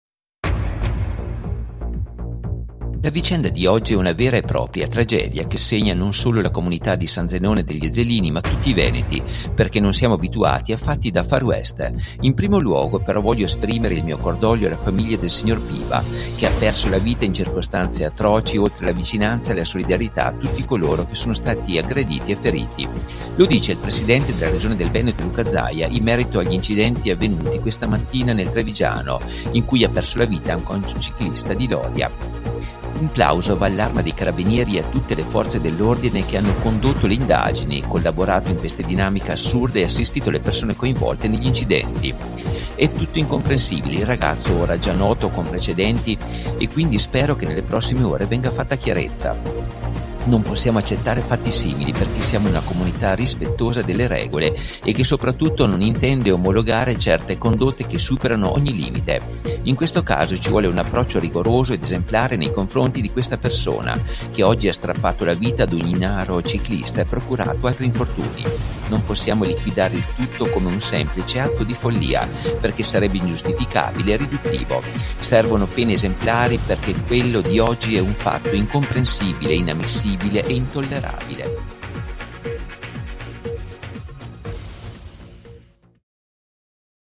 Fuga con tragedia a San Zenone degli Ezzelini, Zaia: “Pena rigorosa e esemplare per il responsabile”
Incidente San Zenone degli Ezzellini. Zaia, “cordoglio ai familiari della vittima e vicinanza ai feriti. Ci vuole un approccio rigoroso ed esemplare perché fatti simili non si ripetano più”